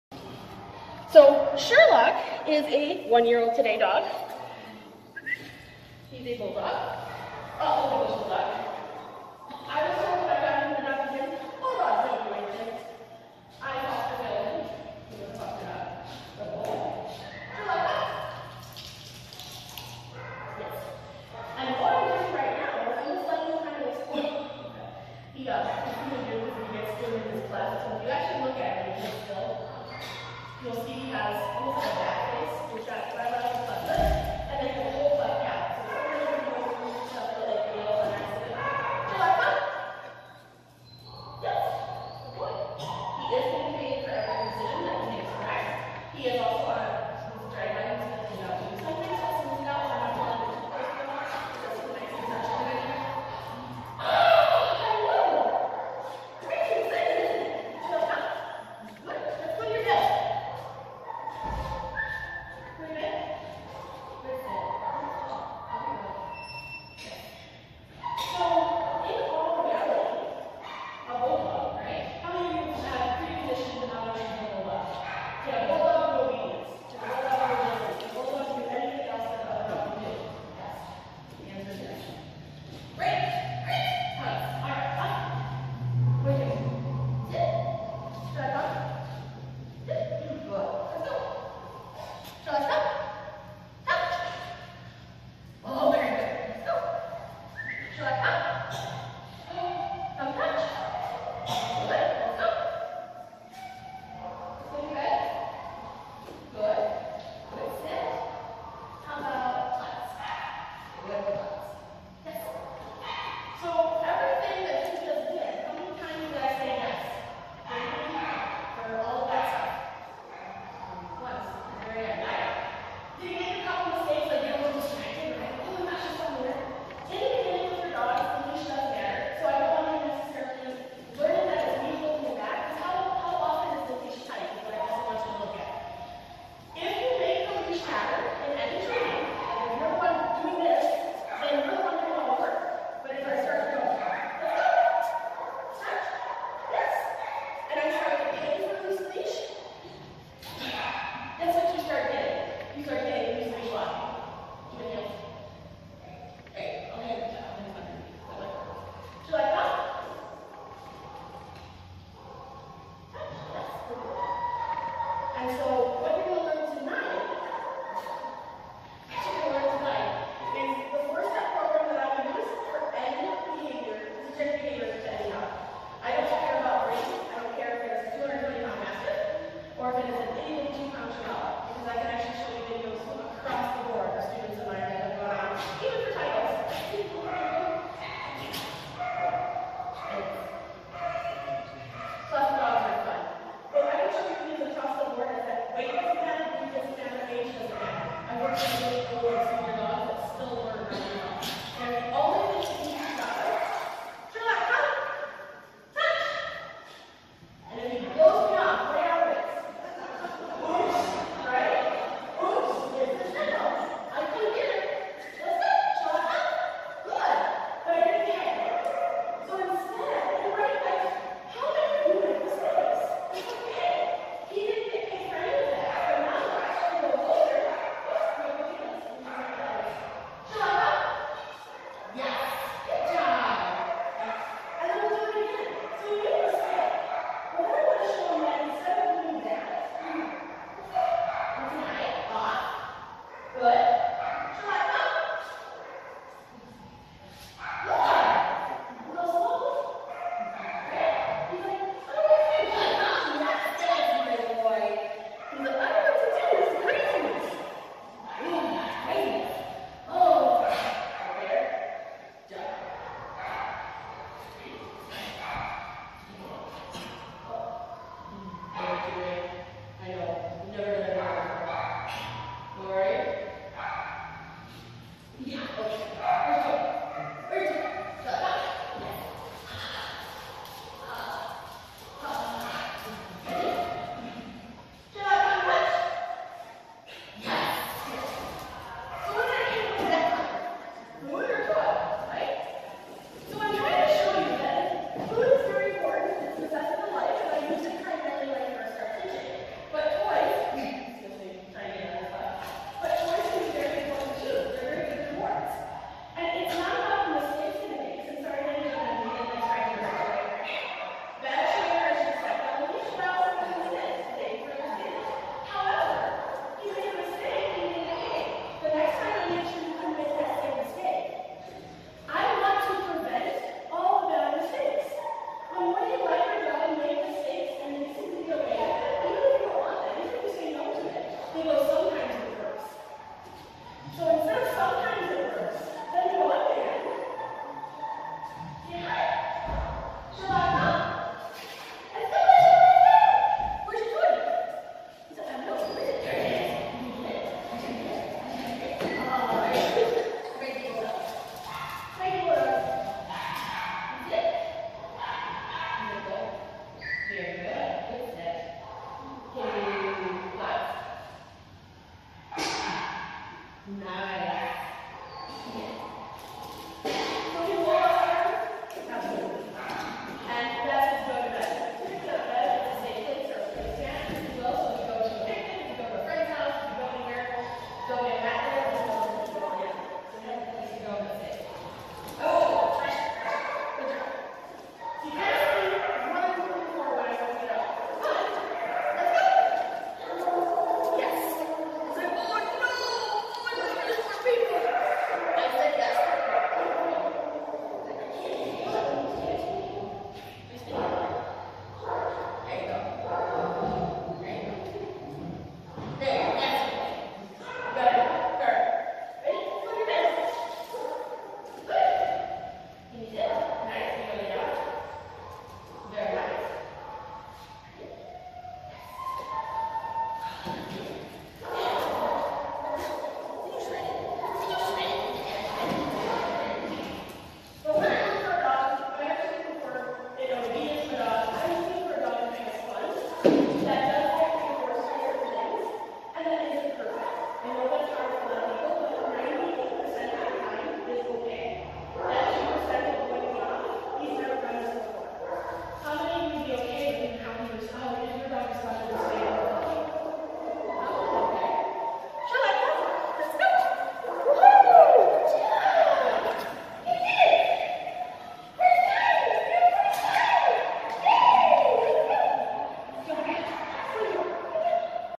Sherlock's Debut as Class Orientation Starter Demo Dog
On his first birthday no less!